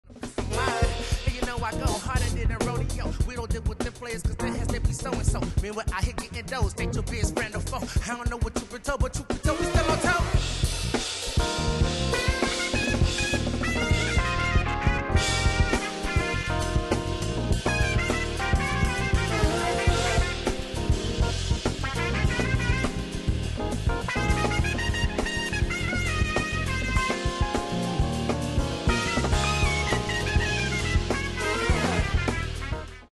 Maurice “MoBetta” Brown ft. Anderson .Paak at Live at Winter Jazzfest 2022 Trailer
Maurice Brown ft. Anderson .Paak at Winter Jazzfest 2022 Trailer for Qwest TV

Also featuring Chelsea Baratz, Parker McAllister, and Takahiro Izumikawa